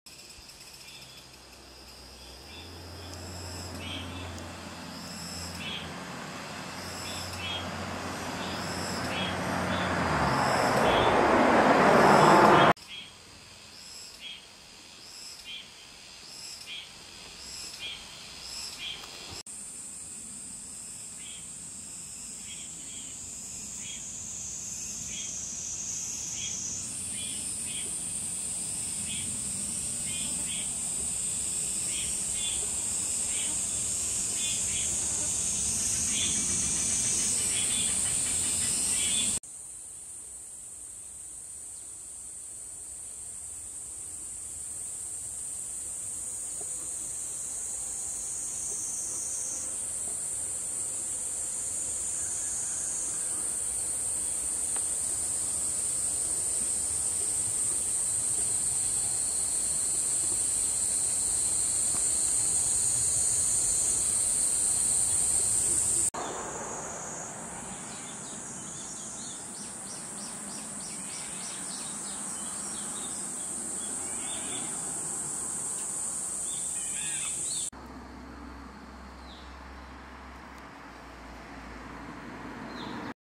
멋쟁이딱정벌레(딱정벌레과)ㅣCarabidae
이름에 멋쟁이가 들어가는 멋쟁이딱정벌레.
어두운 곳에서 보면 검게 보이지만
빛이 있는 곳에서 가까이 보니 멋쟁이란 이름이 잘 어울리는 거 같아요.
자료들을 보니 야행성으로 밤에 나와 곤충이나 지렁이들을 먹는다고 하는데
이 녀석은 밤에 제대로 먹이를 못 먹었는지 낮에 먹이 찾아 먹고 다니는 바람에 보게 되었습니다.
죽은 나비 사체 같은 걸 정리해서 물고 빠르게 이동하는 걸 따라 다녀 보았습니다.
촬영: 2023년 7월 1일 경기도 양평